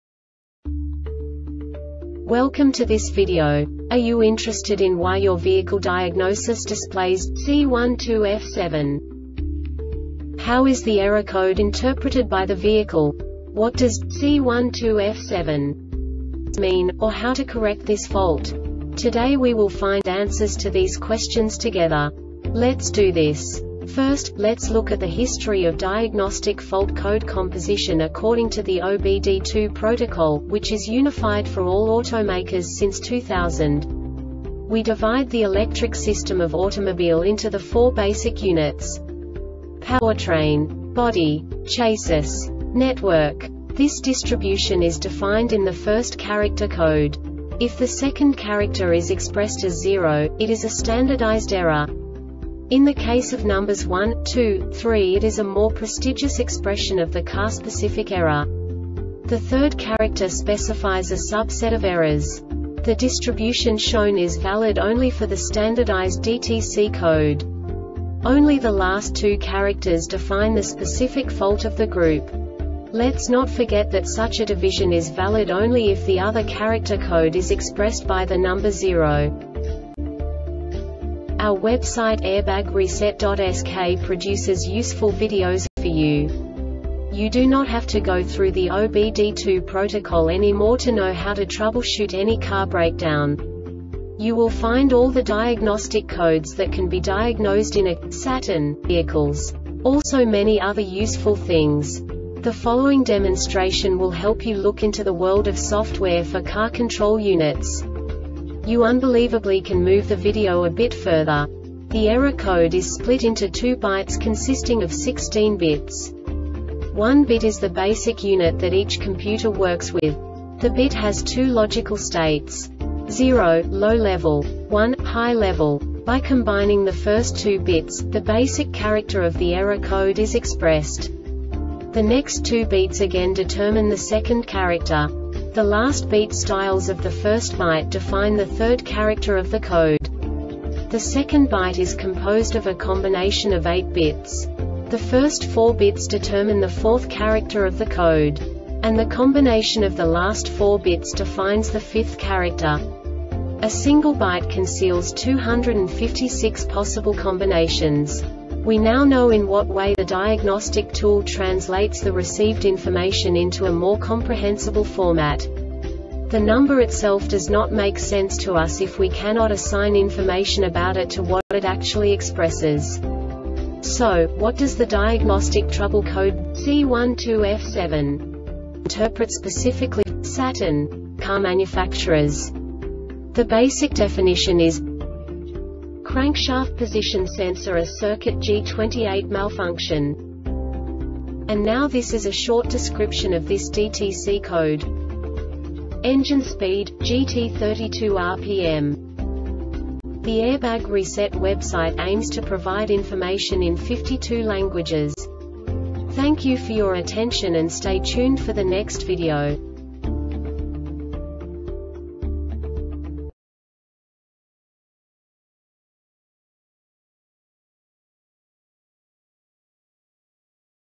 DTC Saturn C12F7 Short Explanation
Contents:
0:21 Basic DTC analysis according to OBD2 protocol standard.
1:48 Insight into programming
2:58 A diagnostically understandable description of the Diagnostic Trouble Code
3:05 Basic error definition

Make:
Saturn
Code:
C12F7
Definition:
Crankshaft Position Sensor (A) Circuit-G28 Malfunction
Description:
Engine speed,&gt;32 RPM